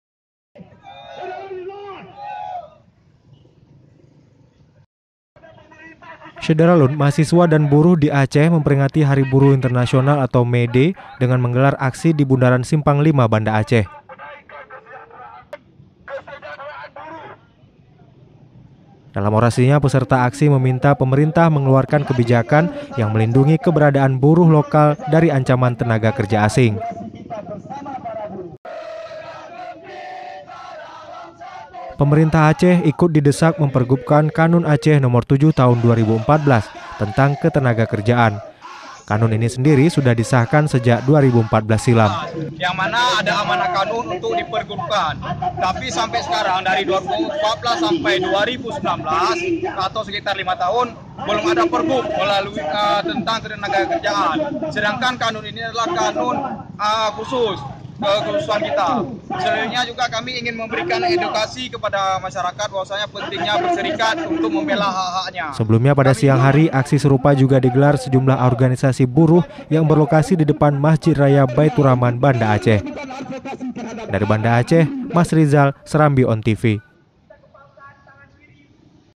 Aksi May Day di Aceh, Pemerintah Aceh Didesak Pergubkan Qanun Aceh Nomor 7 Tahun 2014
SERAMBITV.COM, BANDA ACEH - Mahasiswa dan buruh di Aceh memperingati hari buruh internasional atau May Day dengan menggelar aksi di Bundaran Simpang Lima, Banda Aceh, Rabu (1/5/2019).Dalam orasinya, peserta aksi meminta pemerintah mengeluarkan kebijakan yang melindungi keberadaan buruh lokal dari ancaman tenaga kerja asing.Pemerintah Aceh ikut didesak mempergubkan Qanun Aceh Nomor 7 Tahun 2014 tentang Ketenagakerjaan. Qanun ini sendiri sudah disahkan sejak 2014 silam.Sebelumnya pada siang hari, aksi serupa juga digelar sejumlah organisasi buruh di Aceh, di depan Masjid Raya Baiturrahman Banda Aceh.